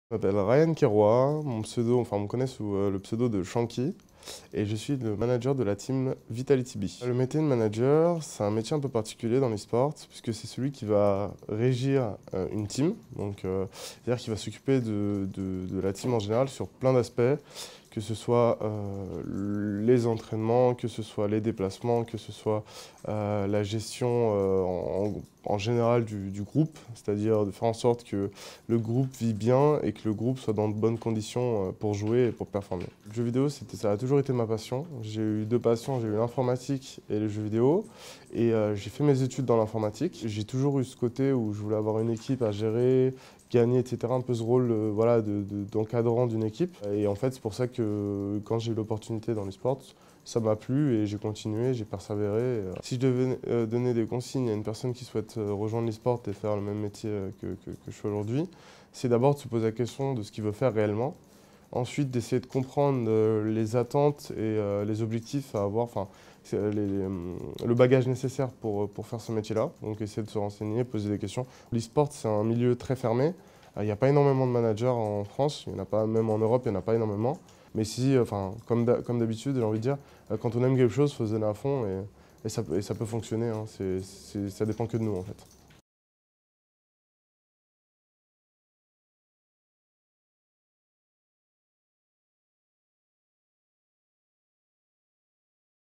Shanky - Interview métier manager esport - Team Vitality
30 itws de professionnels du jeu vidéo et de l'esport en partenariat avec Lyon Esport.